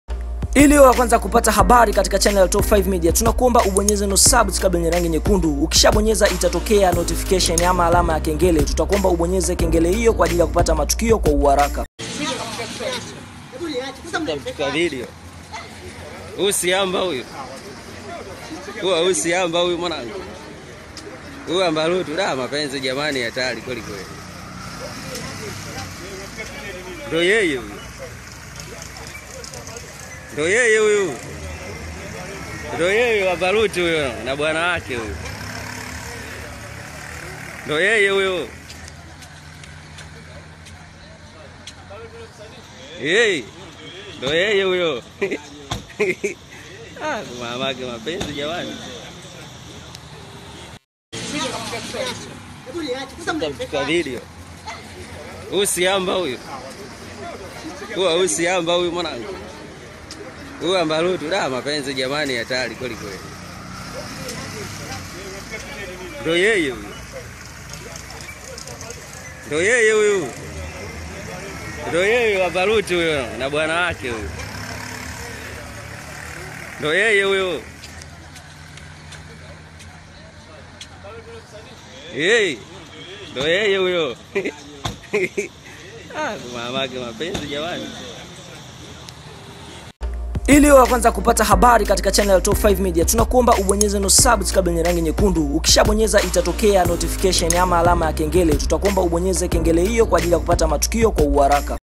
Aibu tena Amber Rutty akamatwa na polisi akiwa amevaa kanga moja mchana kweupe
AmberRutty akamatwa na polisi mchana kweupe akiwa amevaa kanga moja